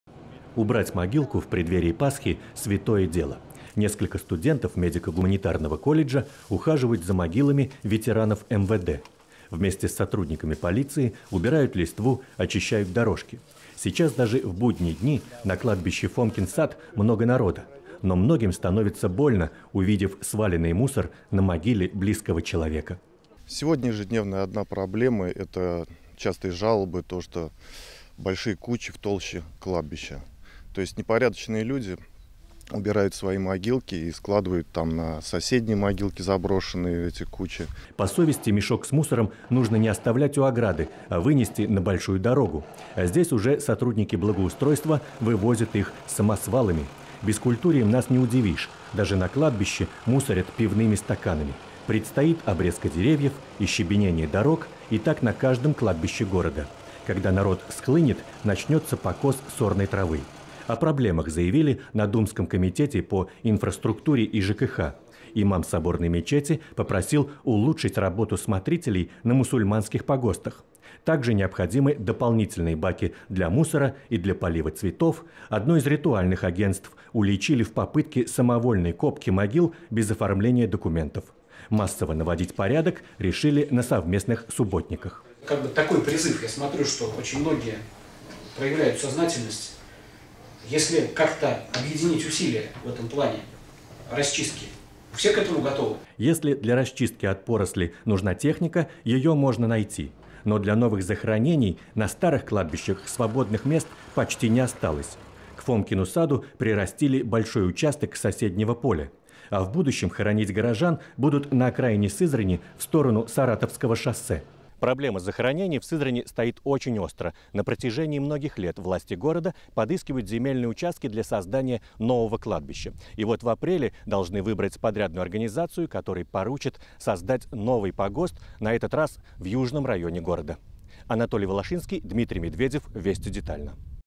Блатным жителям намерены помешать хоронить покойников на кладбищах
Руководителей ритуальной фирмы уличили в самовольной копке могилы, без оформления документов. На кладбища Сызрани перестали пускать автомобили: водители мечутся в гуще людей